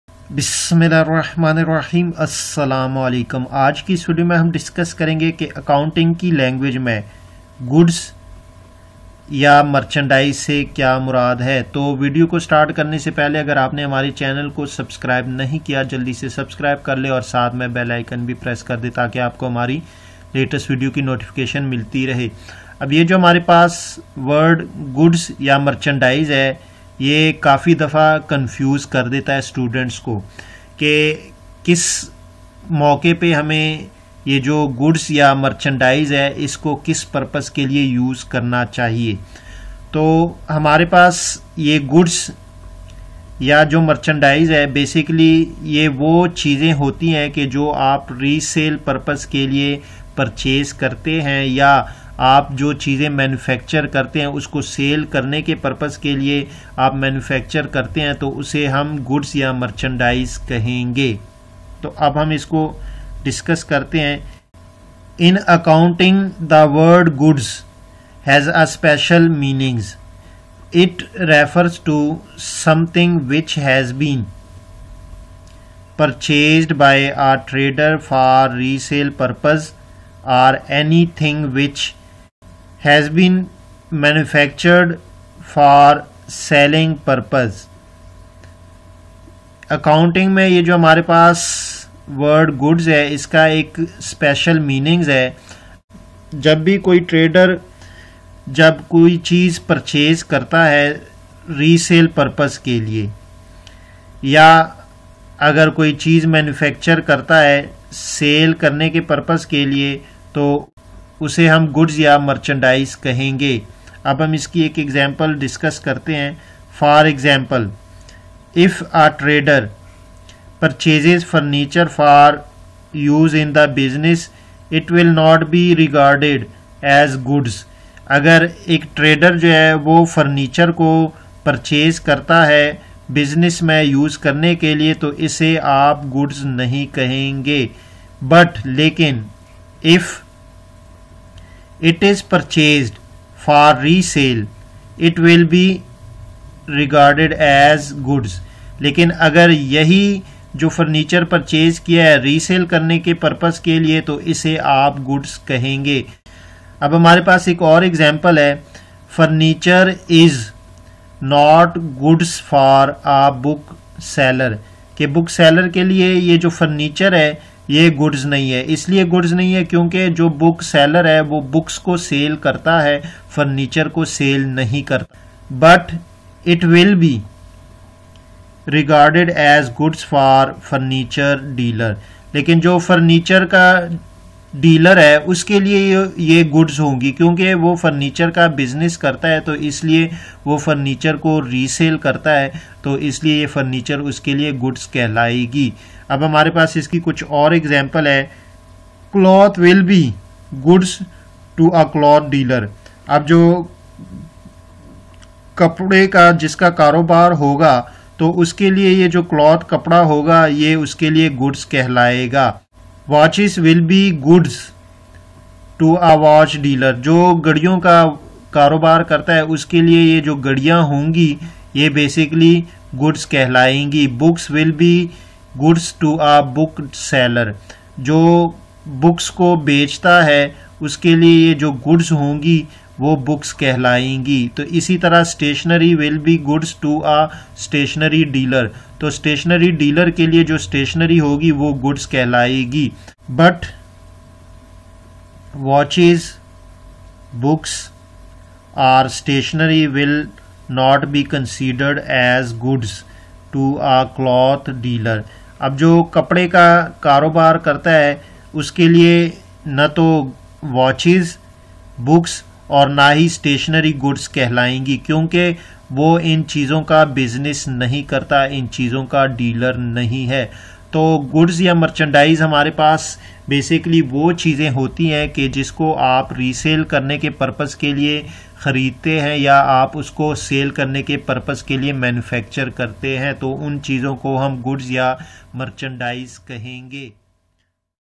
Meaning of Goods or Merchandise in Accounting language by the education forum
How to define goods or merchandise in Hindi | How to define goods or merchandise in Urdu

In this video, We will study about what is goods or merchandise in urdu hindi

Content of this video:

i) What is goods or merchandise?
ii) How to define goods or merchandise?.
iii) Introduction to goods or merchandise.

Definition of Goods or Merchandise in Accounting Language:

In accounting the word goods or merchandise has a special meaning. Those things which has been purchased by a trader for resale purpose or manufactured by trader for selling purpose these things are called goods or merchandise.